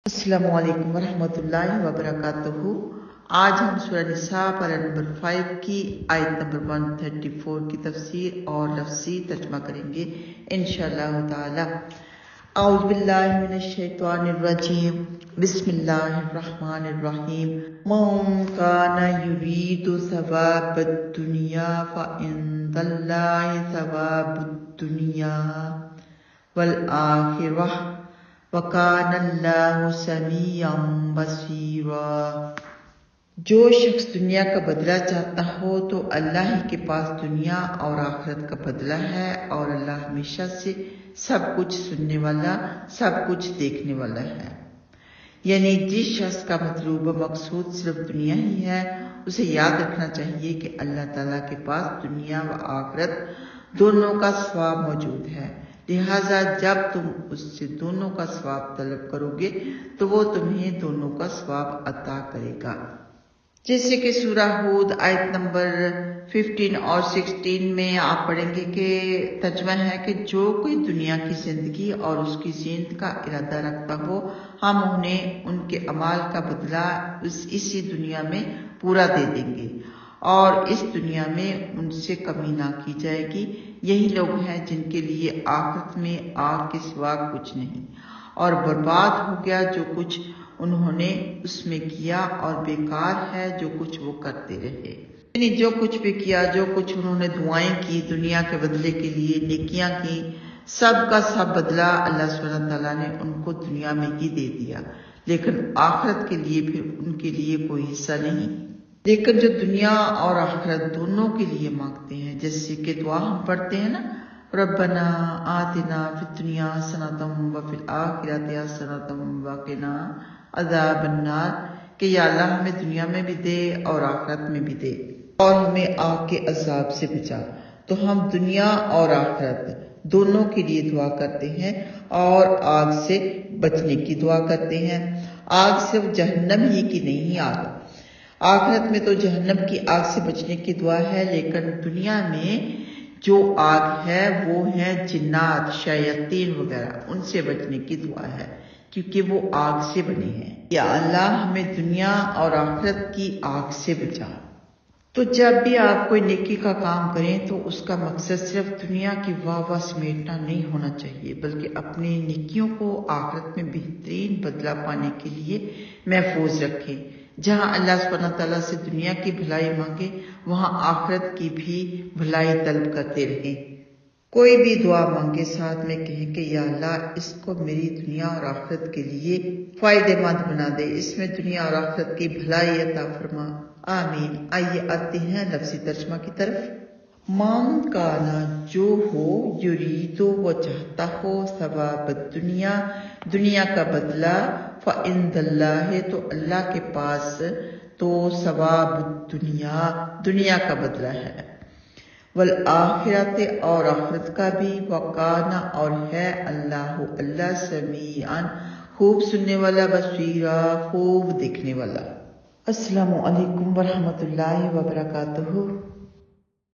Surah An Nisa | Ayat 134 | Quran Tarjuma In Urdu ​⁠​⁠
Translation and details of Quran explained with the concept and object and authentic ahadith related to Quran verses.